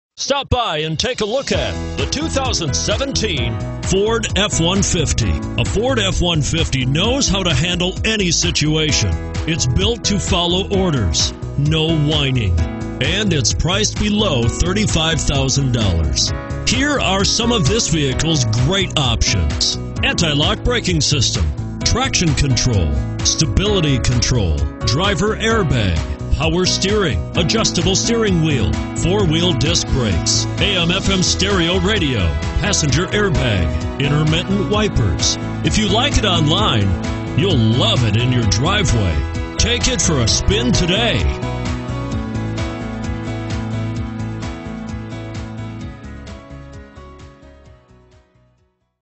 2017 Ford F-150 00T71367
2017 FORD F-150 La Puente, CA
Stock# 00T71367

Pick up this Oxford White 2017 Ford F-150, available today. Featuring Automatic transmission, it has 0 miles. This could be the one you've been searching for.